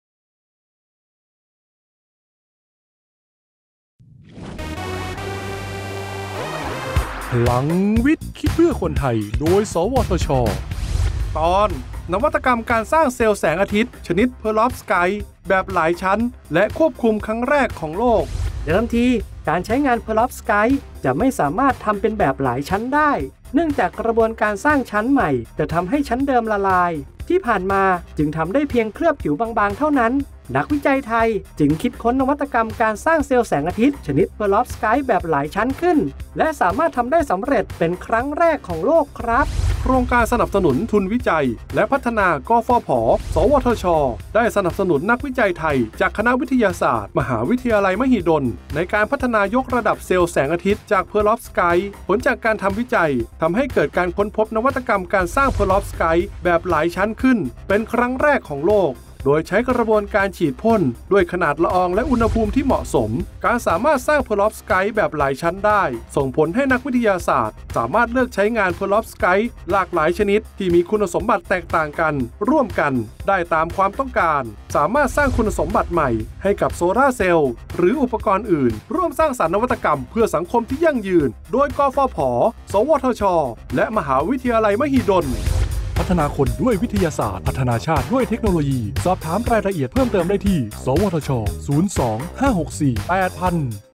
พลังวิทย์ คิดเพื่อคนไทย ตอน นวัตกรรมการสร้างเซลล์แสงอาทิตย์ชนิดเพอรอฟสไกต์ แบบหลายชั้นและควบคุม
โครงการสนับสนุนทุนวิจัยและพัฒนา กฟผ.-สวทช. ได้สนับสนุน นักวิจัยไทยจากคณะวิทยาศาสตร์ มหาวิทยาลัยมหิดล ในการพัฒนายกระดับเซลล์แสงอาทิตย์จากเพอรอฟสไกต์ ผลจากการทำวิจัยทำให้เกิดการค้นพบนวัตกรรม การสร้างเพอรอฟสไกต์แบบหลายชั้นขึ้นเป็นครั้งแรกของโลก โดยใช้กระบวนการฉีดพ่นด้วยขนาดละออง และอุณหภูมิที่เหมาะสม

การสามารถสร้างเพอรอฟสไกต์แบบหลายชั้นได้ ส่งผลให้นักวิทยาศาสตร์สามารถเลือกใช้งานเพอรอฟสไกต์หลากหลายชนิด ที่มีคุณสมบัติแตกต่างกันร่วมกันได้ตามความต้องการ สามารถสร้างคุณสมบัติใหม่ให้กับโซลาร์เซลล์ หรืออุปกรณ์อื่น เช่น เสริมประสิทธิภาพในการใช้งาน และยืดอายุการใช้งานให้ยืนยาวขึ้น